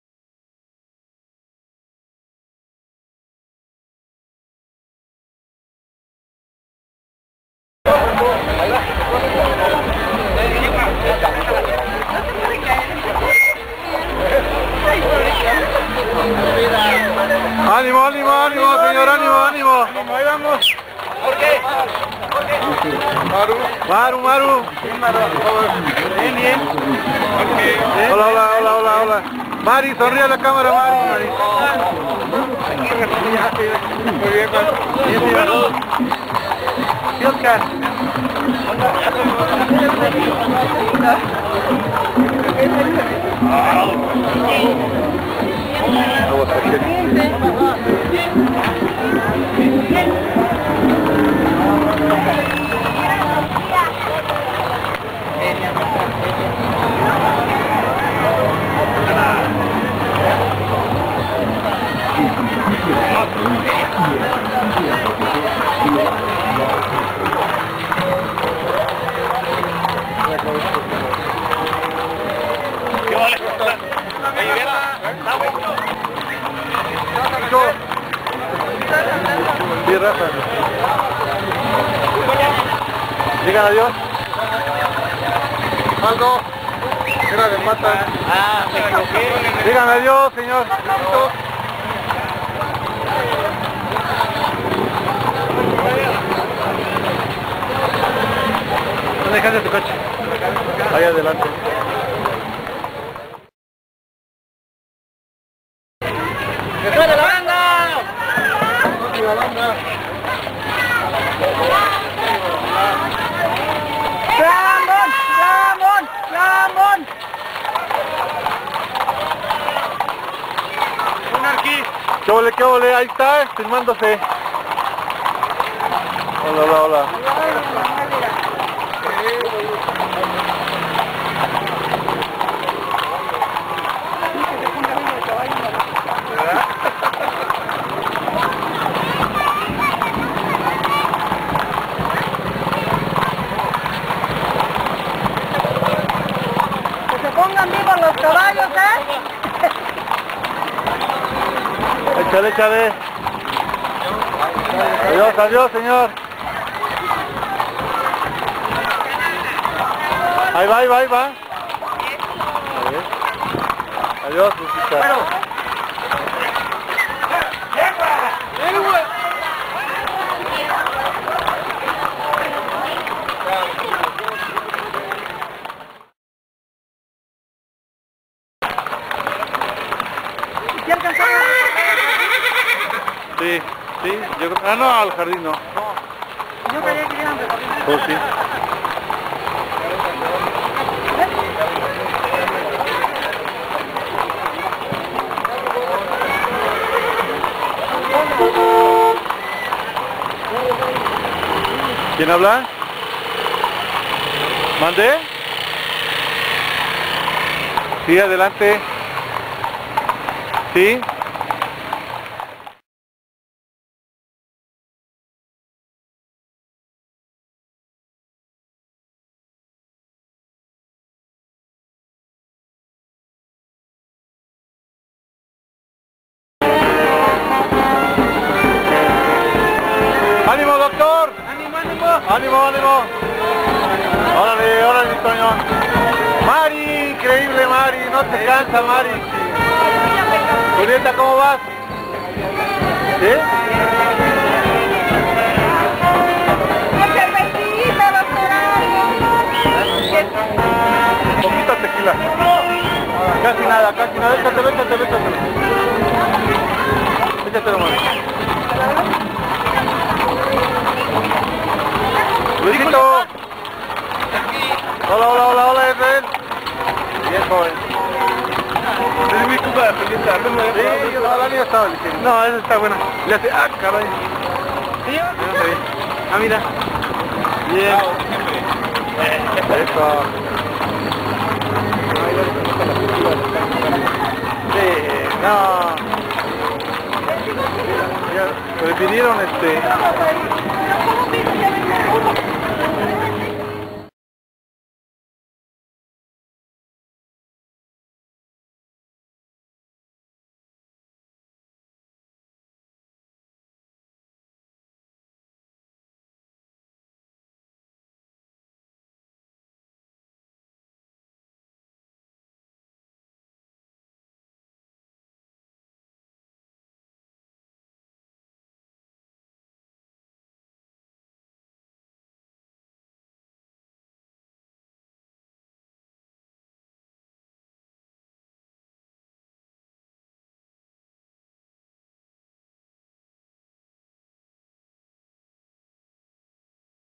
CABALGATA APOYO A CLAMONT
Cabalgata de apoyo a Francisco Javier Fernández Clamont, Candidato del PRI a Presidente Municipal de Cuautitlán, Méx. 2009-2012